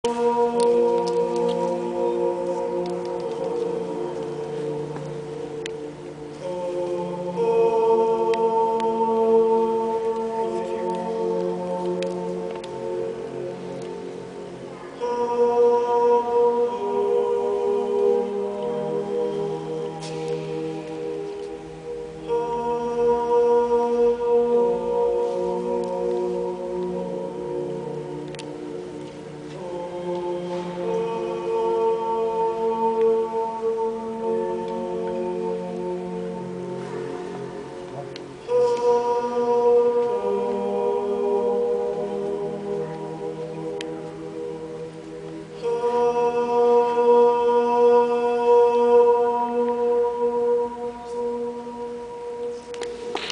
Man Singing Using Only Reverb Inside The Pisa Baptistery In Pisa, Italy.
The Cathedral across from the Leaning Tower of Pisa. I was there for school and a man randomly came in and told everyone to be quiet, then started doing this. It was AMAZING. Turn up your volume to hear the reverb better.